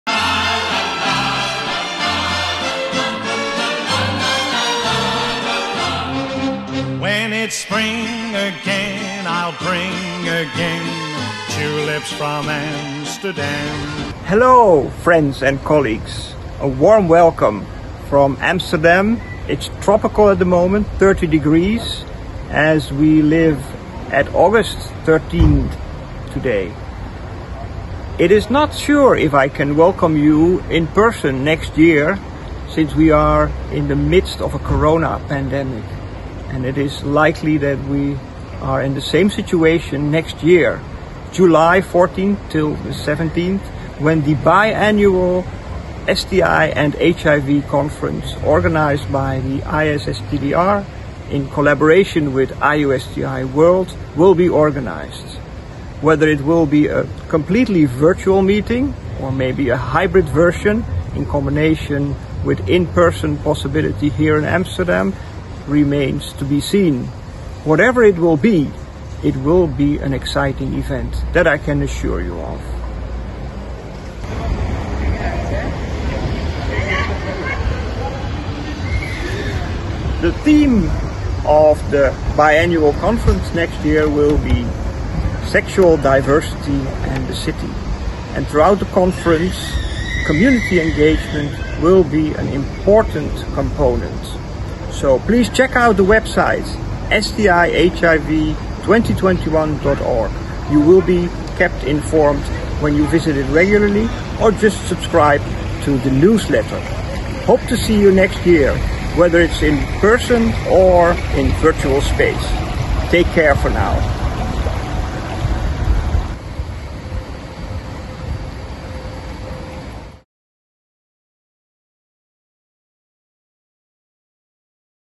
Save the date for the STI&HIV World congress 2021 in Amsterdam STIHIV2021.COM
Welcome to the STI & HIV 2021 World Congress to be held in Amsterdam from 14 to 17 July 2021! This bi-annual meeting dedicated to STI in its broadest sense will be organised by the International Society for STD Research (ISSTDR) in collaboration with the International Union against STI World (IUSTI World), just like earlier editions.

Sexual diversity and the city
“Sexual diversity and the city” is this year’s theme and our mission is to address diversity, health and sexuality in the urban context. Why? Over 60% of the current world population live in urbanised settings, places where a large portion of the STI epidemic is also focussed. At the same time, urban environments offer networks, infrastructures, multidisciplinarity and creativity from which solutions can emerge. Cities can act as giant outdoor labs, experimental sites, research incubators and ideal environments for the implementation of interventions.